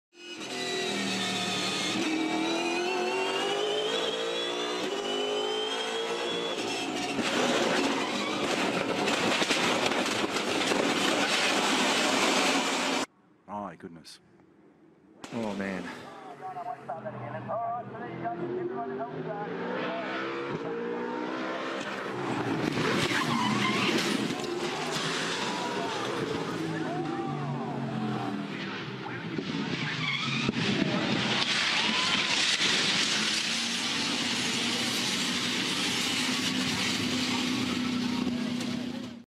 Chaz Mostert Bathurst 1000 V8 Supercars Crash 2015
REIGNING Bathurst 1000 champion Chaz Mostert has broken his left leg and wrist in a horrific crash between the famous Mount Panorama Dipper and Forrest’s Elbow during qualifying for the Bathurst 1000.

Mostert has been air-lifted to hospital in Orange and all track action has been suspended for the day.

The 23-year-old was completing his first ‘fast lap’ of qualifying when he clipped a concrete wall with the front left of his car, sending the right-hand side of his car into the opposite wall.

The car then once again mounted the left-hand wall, sliding along it at high speed and wiping the roof off a track marshals hut before landing on the road at Forrest’s Elbow.

Mostert was taken away from the scene in an ambulance after giving a thumbs up signal to a clearly distressed crowd on the mountain.

The incident ripped doors, wheels and other panels off the #6 Falcon, which now appears a complete write off.